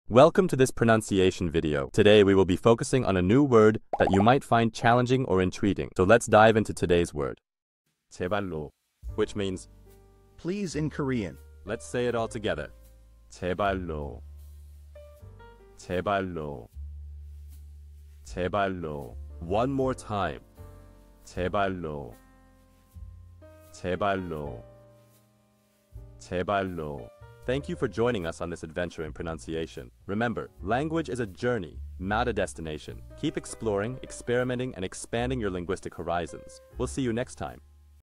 How to pronounce jebalro | 제발로 (Please in Korean)
Master the Pronunciation of 'jebalro | 제발로' - which means : Please in Korean 📝 with @PronunciationKorean 🎓🗣️ - [Your Guide to Korean]

Learn to pronounce 'jebalro' perfectly in Korean with our easy and engaging tutorial by @PronunciationKorean! 📖📽️ In this comprehensive video, we provide you with native speaker examples 🗣️👂 and phonetic breakdowns 🎵 to ensure you get the pronunciation just right.